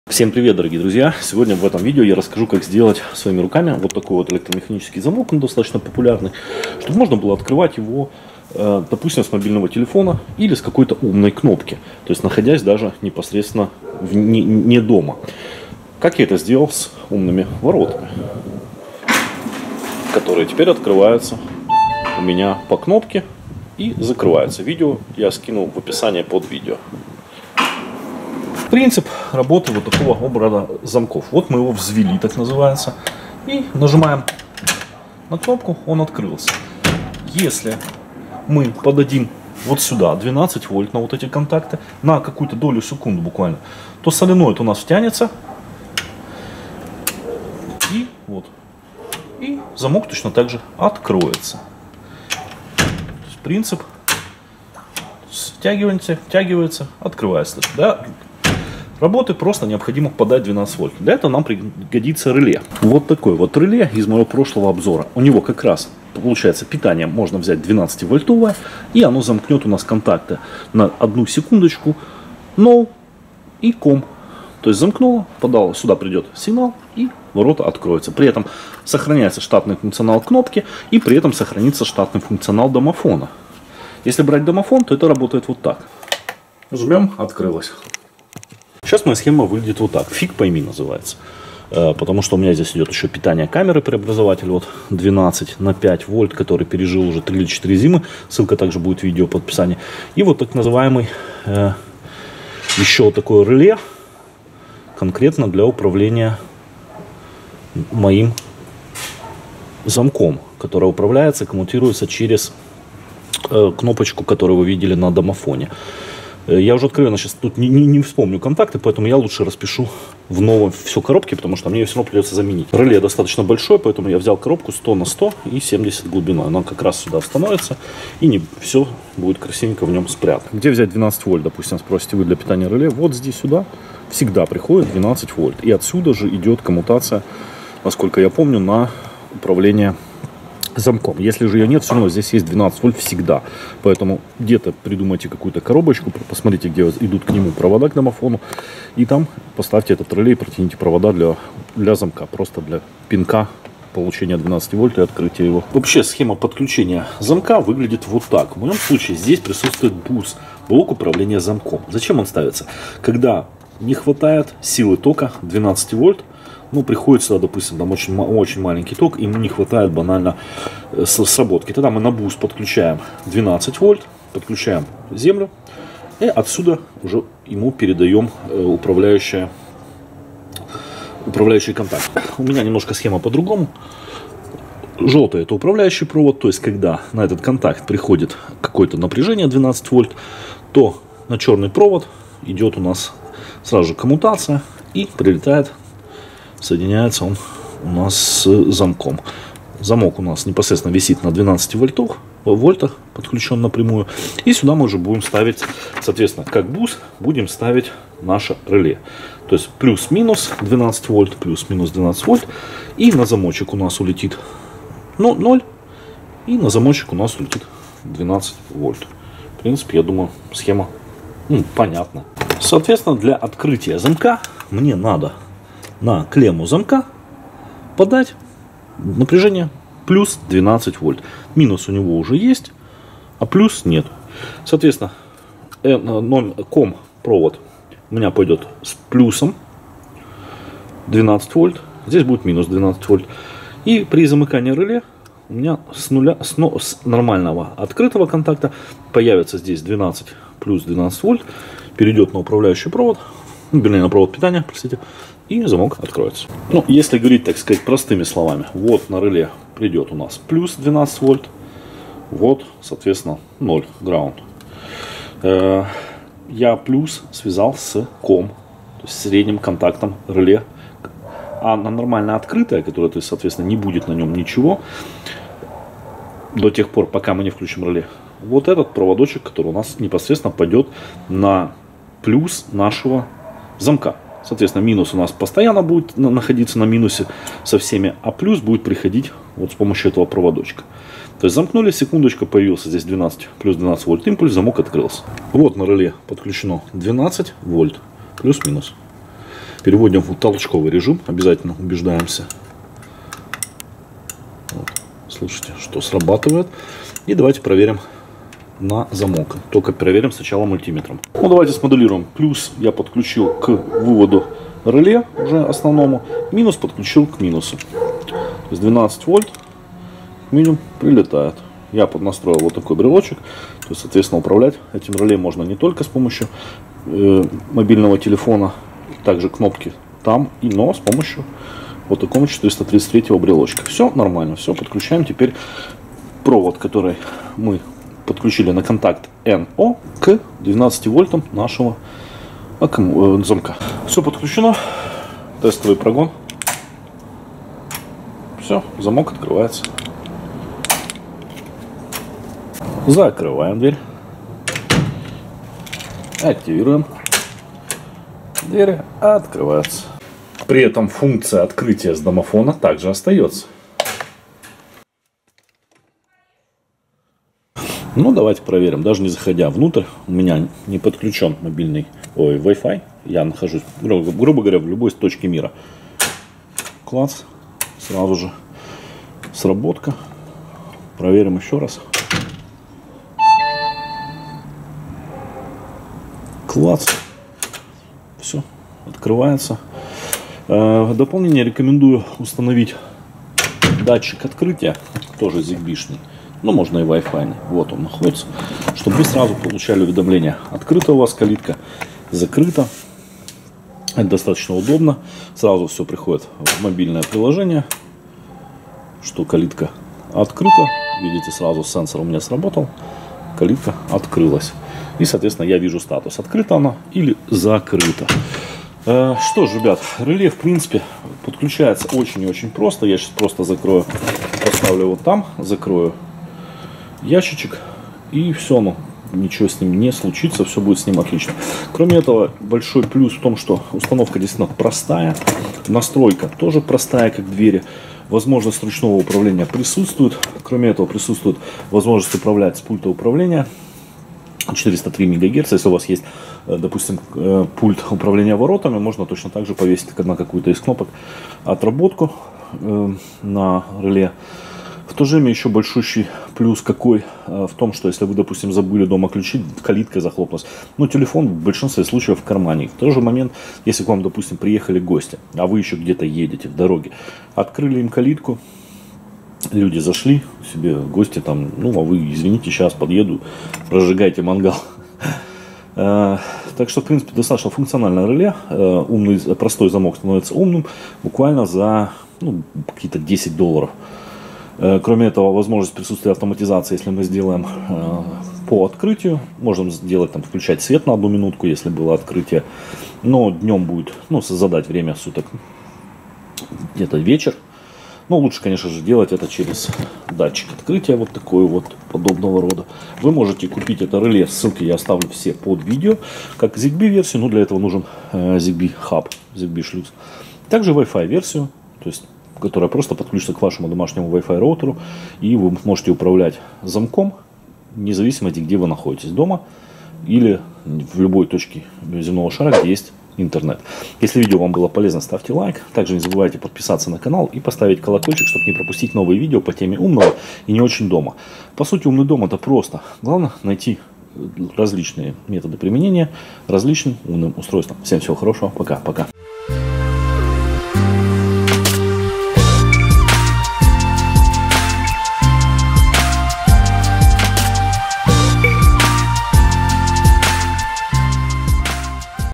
DIY Умное Реле для калитки: Открывай замок просто с телефона или пульта
🔓✨ Как превратить обычную калитку и электромеханический замок в часть умного дома! В этом DIY руководстве я покажу вам, как легко и просто можно модернизировать стандартный электромеханический замок видео домофона, чтобы управлять им через мобильный телефон. Забудьте о ключах и мучительном поиске их в кошельке или кармане – теперь ваш мобильный телефон станет вашим надежным ключом к дому! Будь то WiFi или ZigBee реле с сухими контактами - я расскажу обо всех этапах процесса, начиная от выбора компонентов до полной настройки системы.
⚙️ DIY: Шаг за шагом вы увидите процесс установки и настройки умного реле, подключения его к замку и интеграции с вашим смартфоном.
📱🌍 Управление доступом из любой точки мира: Благодаря интернет соединению, отныне вы сможете открывать и закрывать калитку не вставая с кресла или даже находясь в другой стране. А благодаря наличию датчика открытия - можно сразу получать уведомление о состоянии калитки, закрыта она или открыта - очень просто и удобно!
🔑 Прощай, связка ключей! Ваш новый ключ - это смартфон. Это не только удобно, но и повышает безопасность вашего дома. А еще можно открывать замок пультом 433 MHz. Но так же не забываем, что мы только добавляем возможность дополнительного открытия замка. Все остальные возможности, ключи, кнопка или видео домофон остаются актуальными для открытия замка.
💡 Полезные советы и рекомендации по безопасности: Я поделюсь с вами лучшими практиками и важными моментами безопасности, которые необходимо учесть при "умной" модернизации замка.

00:28 – 02:35 Вступление
02:36 – 03:41 Общая схема подключения реле к замку и домофону
03:41 – 06:07 Подключение реле с сухими контактами
06:07– 09:12 Выводы